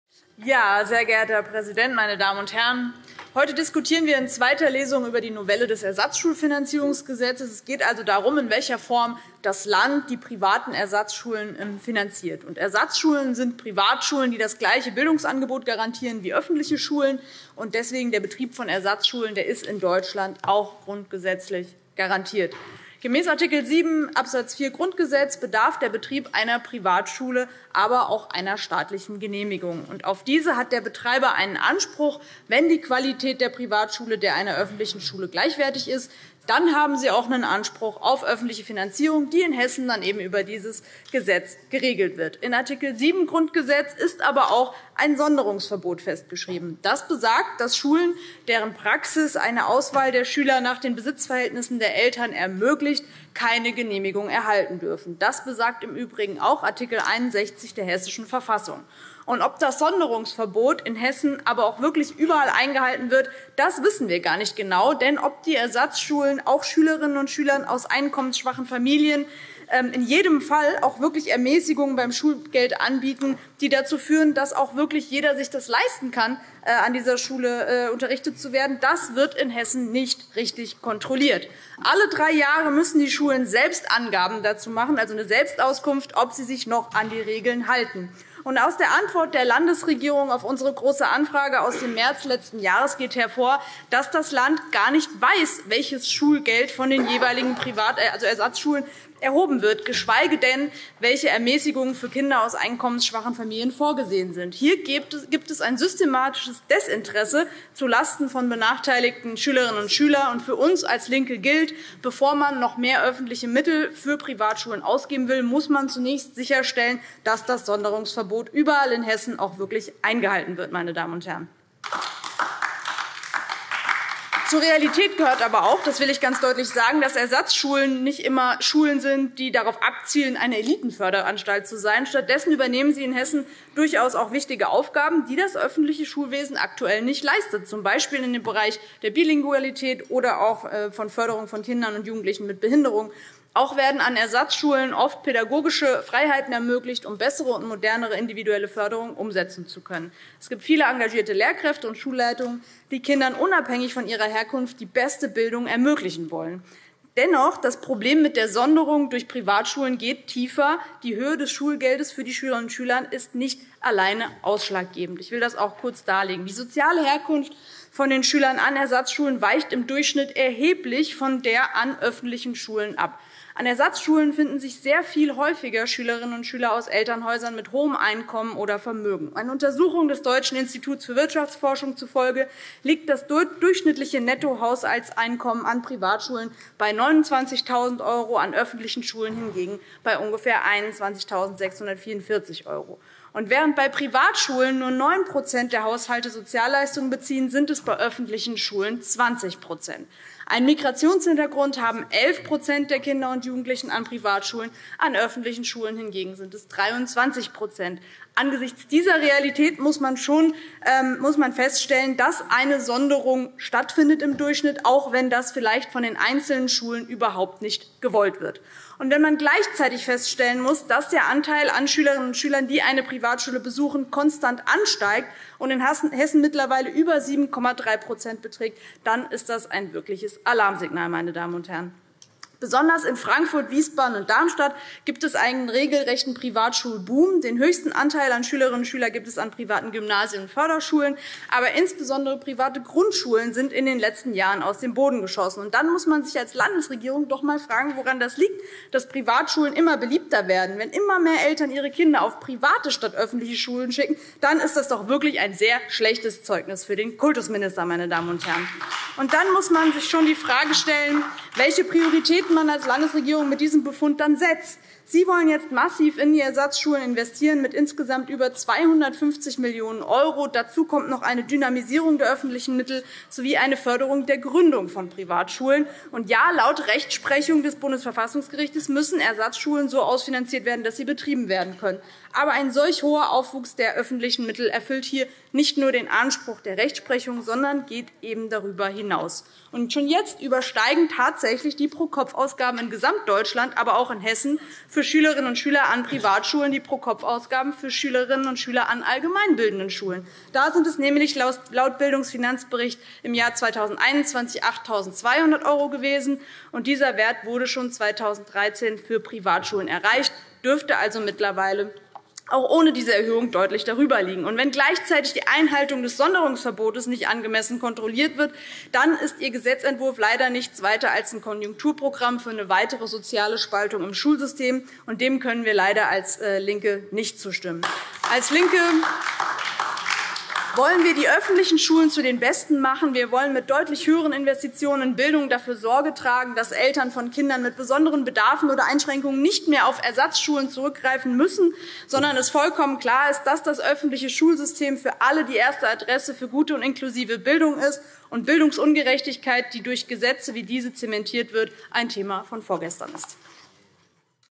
Elisabeth Kula zum Gesetzentwurf zur Änderung des Ersatzschulfinanzierungsgesetzes
In seiner 140. Plenarsitzung am 19. Juli 2023 diskutierte der Hessische Landtag über den Gesetzentwurf der Landesregierung in Zweiter Lesung zur Änderung des Ersatzschulfinanzierungsgesetzes. Dazu die Rede unserer Fraktionsvorsitzenden und bildungspolitischen Sprecherin Elisabeth Kula.